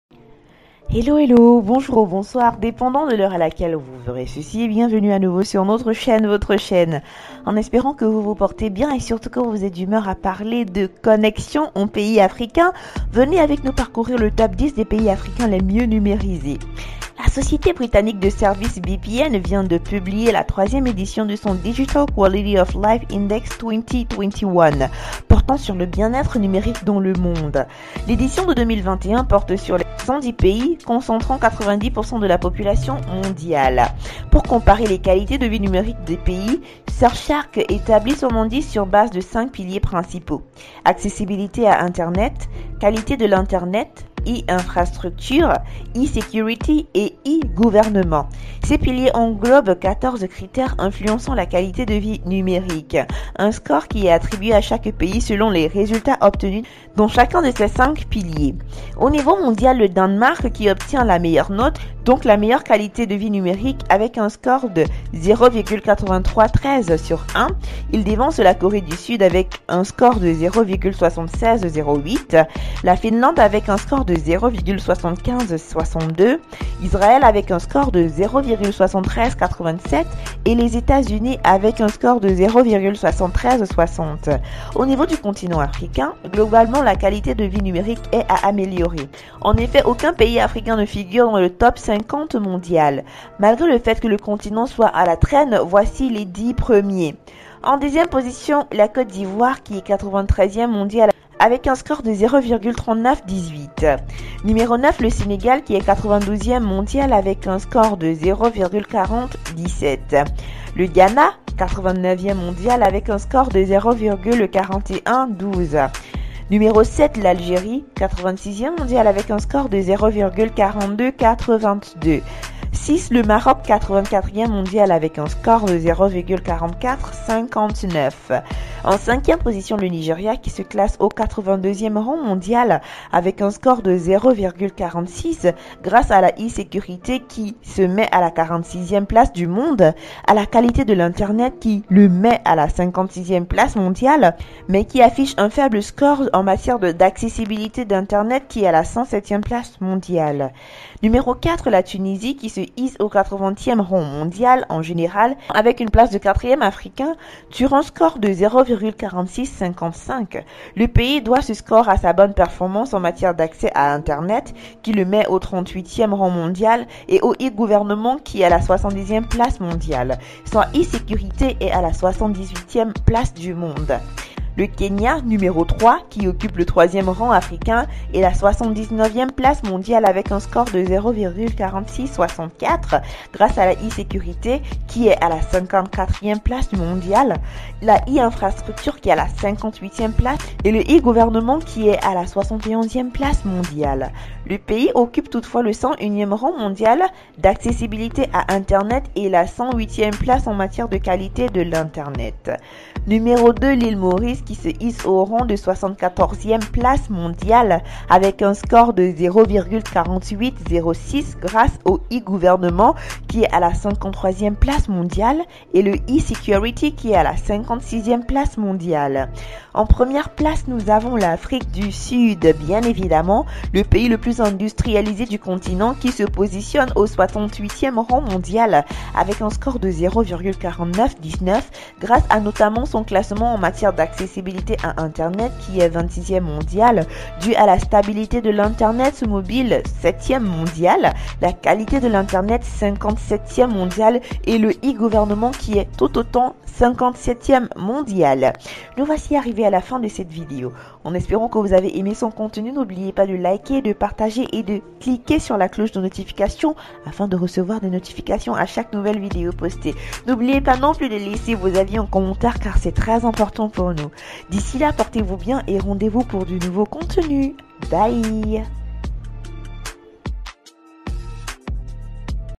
TOP 10 DES PAYS AFRICAINS LES MIEUX NUMÉRISÉS .
La société britannique de services VPN vient de publier la 3e édition de son "Digital quality of live
index 2021", portant sur le bien-être numérique dans le monde. L’édition de 2021 porte sur 110 pays
concentrant 90% de la population mondiale.
Pour comparer les qualités de vie numérique des pays, Surfshark établit son indice sur la base de 5
piliers principaux: accessibilité internet, qualité de l’internet, e-infrastructures, e-sécurité et e-
gouvernement. Ces piliers englobent 14 critères influençant la qualité de vie numérique. Un score est
attribué à chaque pays, selon les résultats obtenus dans chacun des cinq piliers. Au niveau mondial,
c’est le Danemark qui obtient la meilleure note, donc la meilleure qualité de vie numérique avec un
score de 0,8313 sur 1. Il devance la Corée du Sud (0,7608), la Finlande (0,7562), Israël (0,7387) et
Etats-Unis (0,7360). Au niveau du continent africain, globalement, la qualité de vie numérique est à
améliorer. En effet, aucun pays africain ne figure dans le top 50 mondial. Malgré le fait que le
continent soit à la traîne, voici les 10 meilleurs !!!
10) Côte d’Ivoire (93e) avec un score de 0,3918.
9) Sénégal (92e) avec un score de 0,4017
8) Ghana (89e) avec un score de 0,4112
7) l’Algérie (86e) avec un score de 0,4282
6) Maroc (84e) avec un score de 0,4459
5) Le Nigeria : qui se classe au 82e rang mondial avec un score de 0,46 grâce à l’e-sécurité (46e), à la
qualité de l’Internet (56e), mais qui affiche un faible score en matière d’accessibilité à l’internet (107e
mondial).
4) La Tunisie : se hisse au 80e rang mondial et 4e africain avec un score de 0,4655. Le pays doit ce
score à sa bonne performance en matière d’accès à l’internet (38e mondial), à l’e-gouvernement
(77e) et l’e-sécurité (78e).
3) Le Kenya :  occupe le 3e rang africain et le 79e mondial avec un score de 0,4664 grâce à l’e-sécurité
(54e), l’e-infrastructure (58e) et l’e-gouvernement (71e). Le pays occupe toutefois le 101e rang
mondial d’accessibilité à l’internet et le 108e en matière de qualité de l’internet.
2) L'ILE Maurice :  qui se hisse au 74e mondial avec un score de 0,4806 grâce à l’e-gouvernement
(53e) et l’e-sécurité (56e).
1) L'Afrique Du Sud :   le pays le plus industrialisé du continent, qui se positionne au 68e rang mondial
avec un score de 0,4919, grâce notamment à son classement en matière d’accessibilité à l’internet
(26e mondial) dû à la stabilité de l’internet mobile (7e mondial), la qualité de l’internet (57e) et le e-
gouvernement (57e).